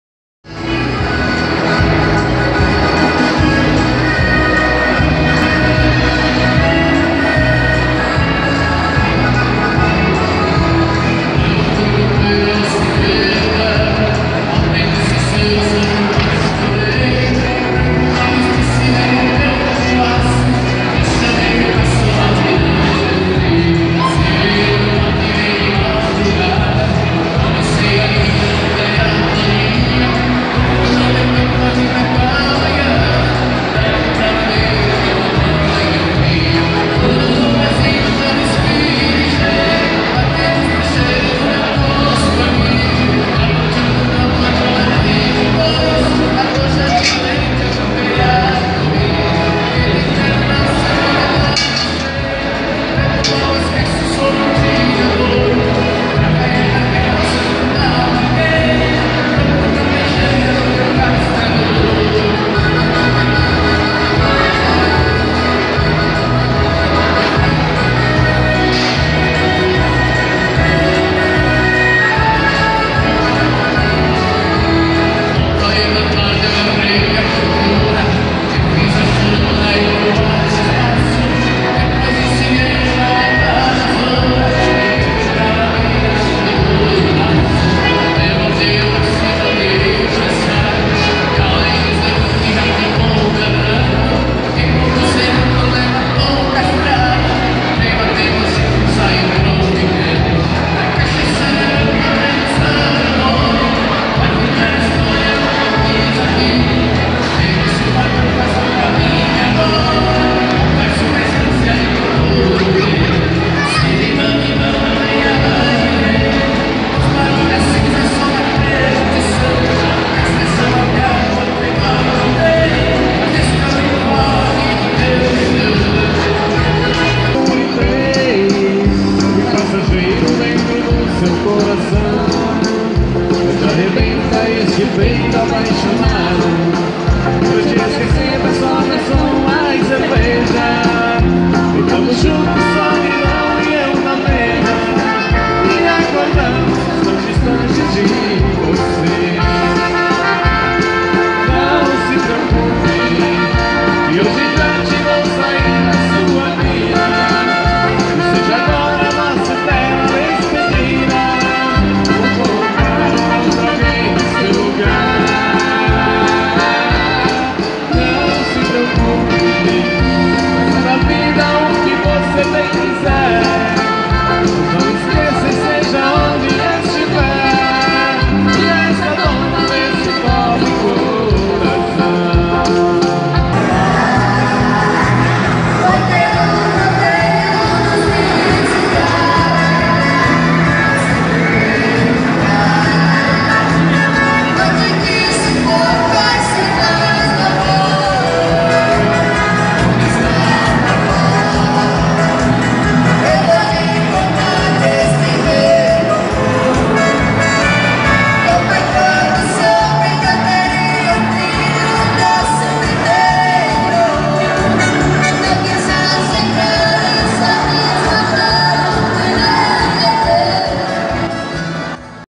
Gruppo Italiani in Brasile na Linha Gramado
Gruppo Italiani in Brasile na Linha Gramado, Rondinha-RS. Veja todas as fotos em nosso blog